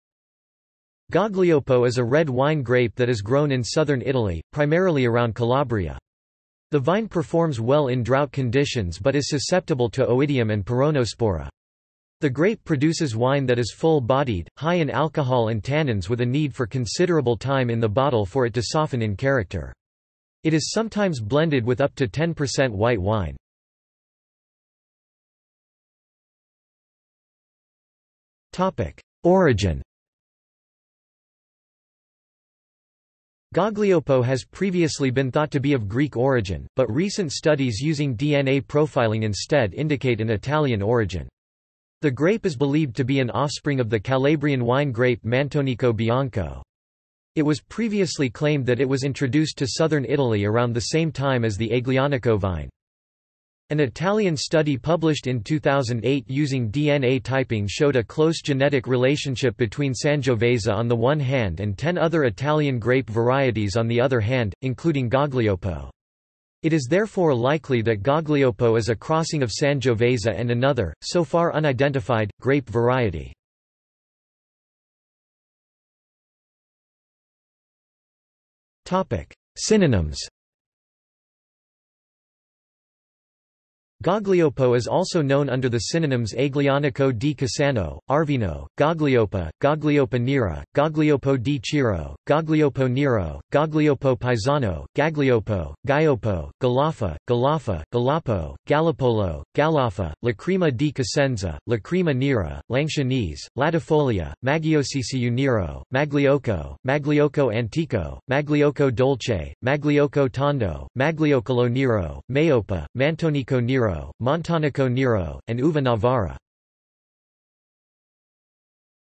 Gaglioppo | Wikipedia audio article
This is an audio version of the Wikipedia Article:
Gaglioppo

00:00:24 undefined
00:01:15 undefined

- Socrates

SUMMARY
Gaglioppo is a red wine grape that is grown in southern Italy, primarily around Calabria. The vine performs well in drought conditions but is susceptible to oidium and peronospora. The grape produces wine that is full-bodied,  high in alcohol and tannins with a need for considerable time in the bottle for it to soften in character. It is sometimes blended with up to 10% white wine.